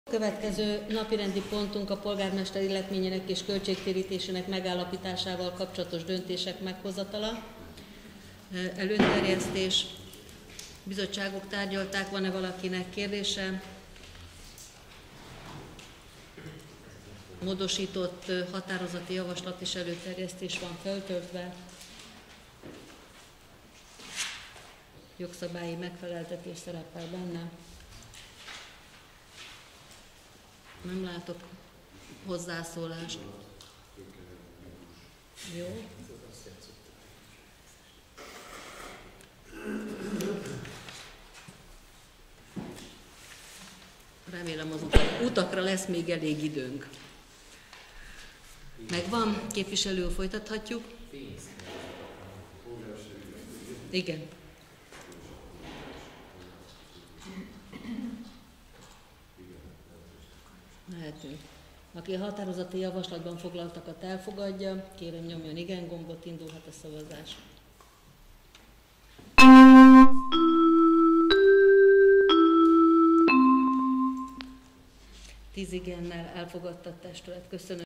Abony Képviselő-Testületi ülése 2017. január 26. 16 napirendi pont
A polgármester illetményének és költségvetésének megállapításával kapcsolatos döntések meghozatala